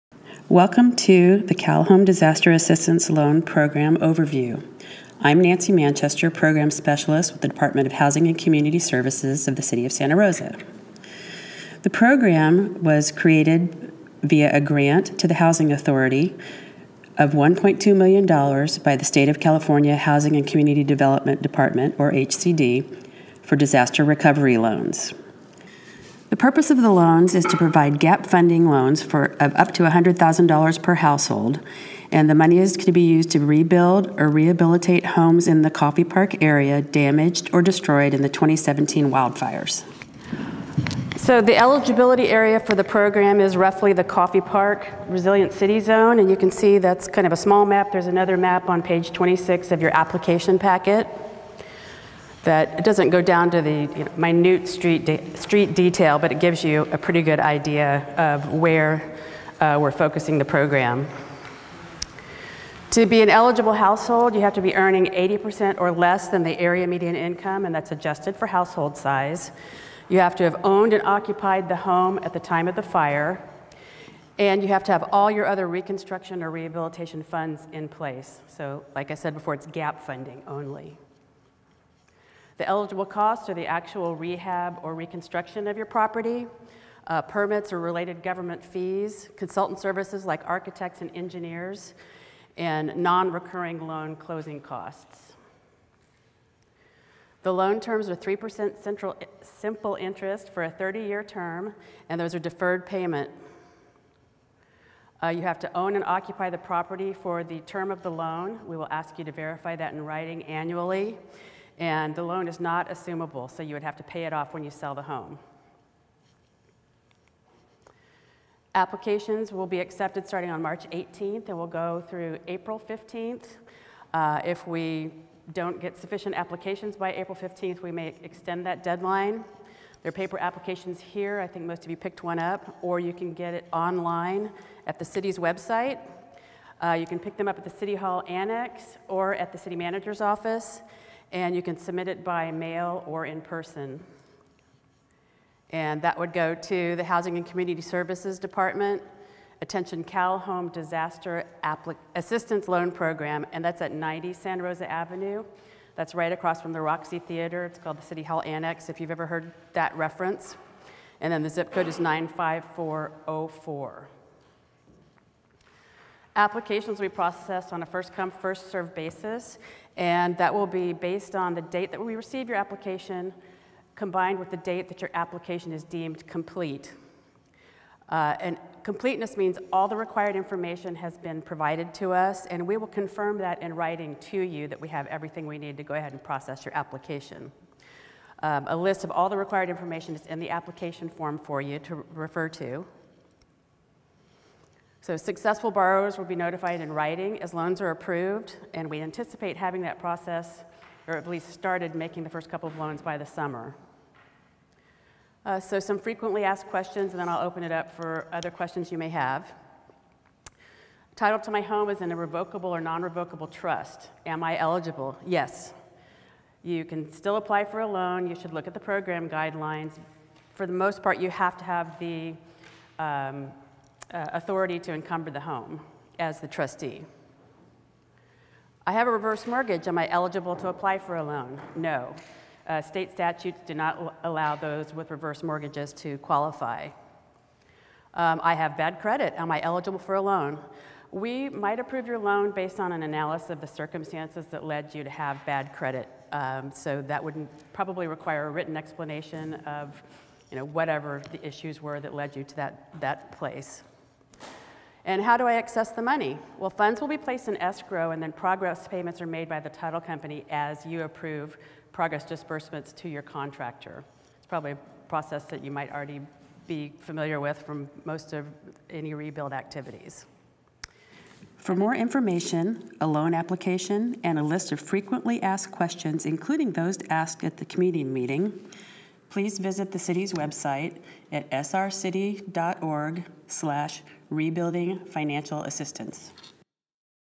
CalHome Disaster Loan Assistance Program for Coffey Park Homeowners
After the 2017 wildfires, the Housing Authority of the City of Santa Rosa applied for and was awarded a $1.2 million CalHome Disaster Assistance grant from the California Department of Housing and Community Development. To best utilize these funds, the Housing Authority is offering low-interest, deferred payment loans as gap financing for the reconstruction or rehabilitation of income-qualified, owner‐occupied, single‐family homes located in the Coffey Park area. Eligible Coffey Park area homeowners whose homes were damaged or destroyed in the October 2017 wildfires and who have not yet begun to rebuild will be able to apply for gap funding of up to $100,000.
This video is a recording of the informational community meeting that was held on Monday, March 11th from at Finley Community Center to explain the program in detail.

Additional program information, including frequently asked questions and the loan application, can be found